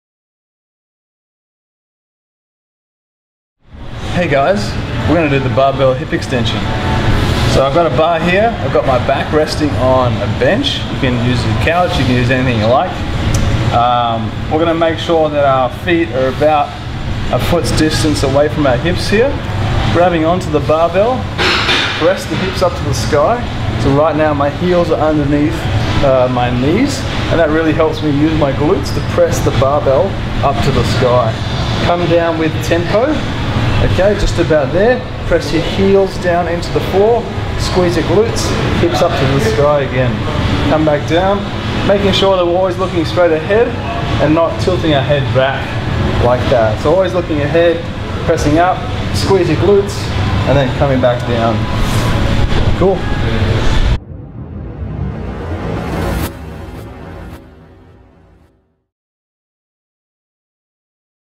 5EW Exercises: BB Hip Extension
When doing BB Hip Extension make sure that you squeeze your glutes when your body goes up.